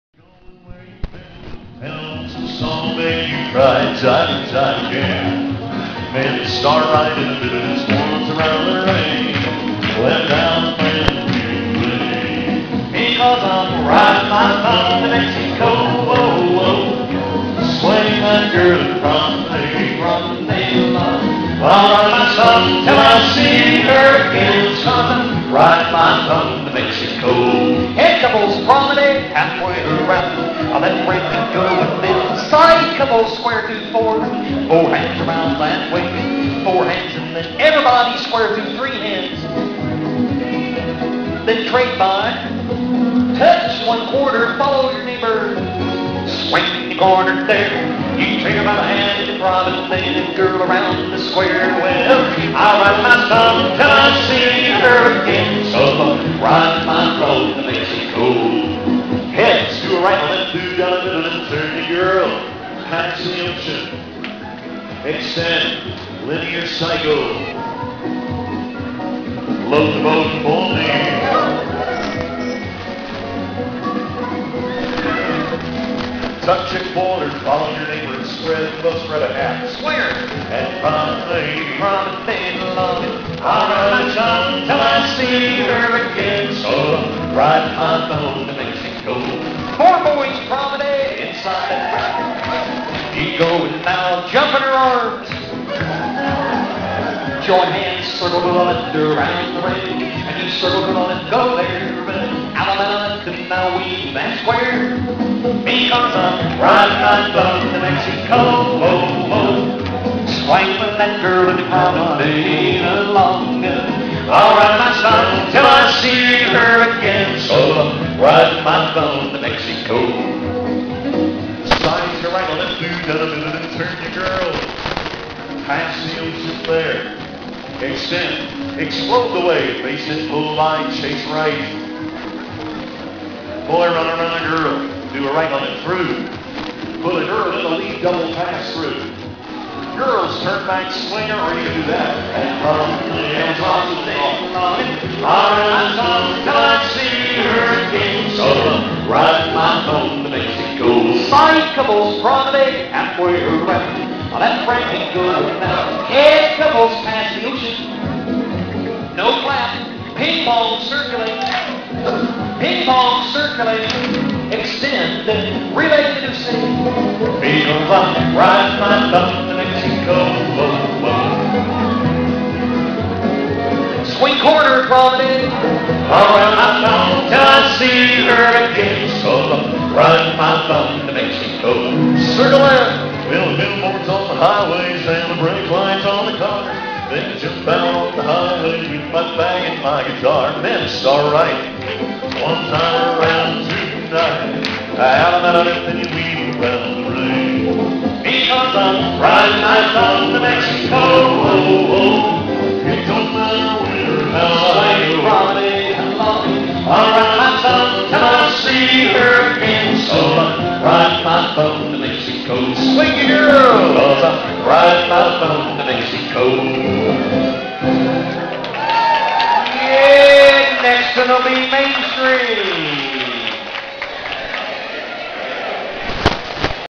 Square Dance in Denver, Colorado in 2011 with Tom Roper & Mike Sikorsky square dance callers
May Madness in Golden, Colorado